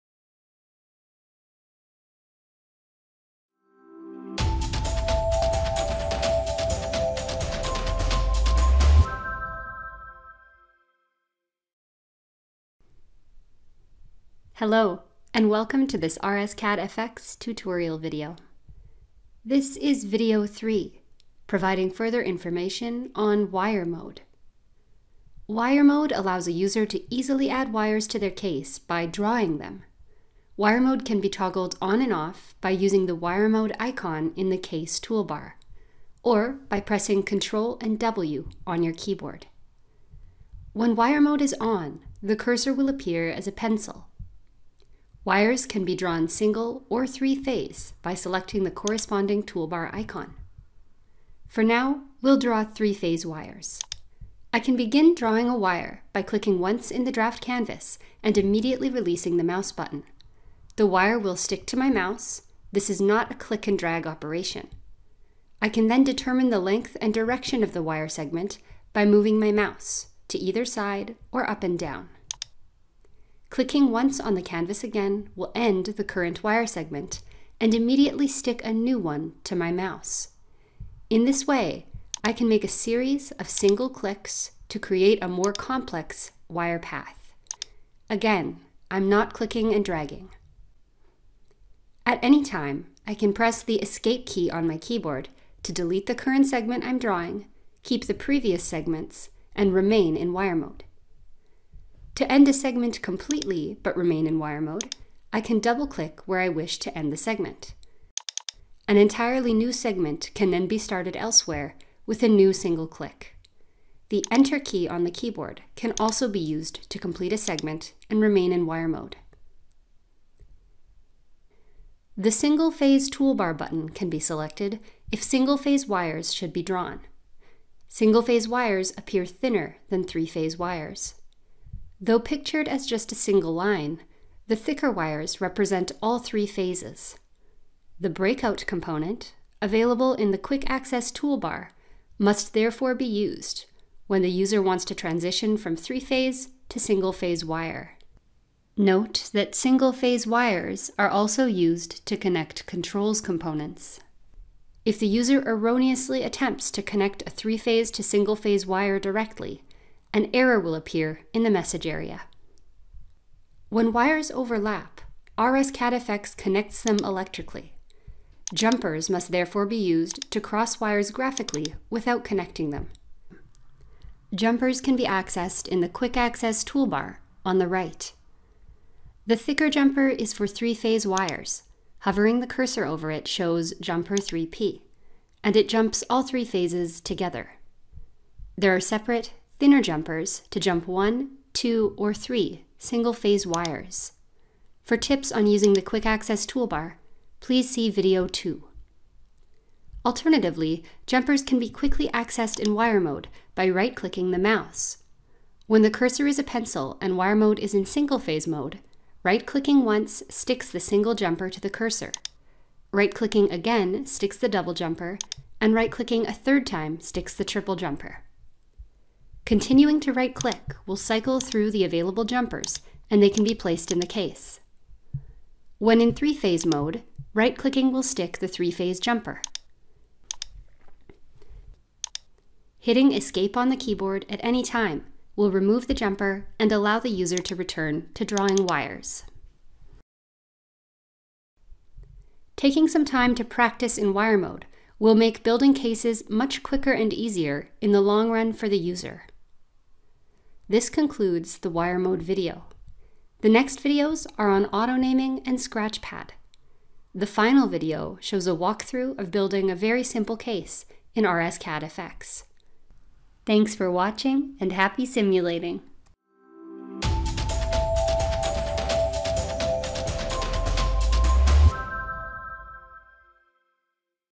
RSCAD FX Demo Video 3 - Wire Mode
Meet RSCAD FX: the next evolution in simulation software by RTDS Technologies. RSCAD FX is the new and improved software for the RTDS Simulator, delivering the power industry’s most advanced real-time modelling library, now with enhanced functionality, features, and a modern look and feel. This series of videos is intended to guide users through the updated RSCAD FX graphical user interface and explain new features. The Quick Start Guide, available in RSCAD FX, provides an accompanying written explanation.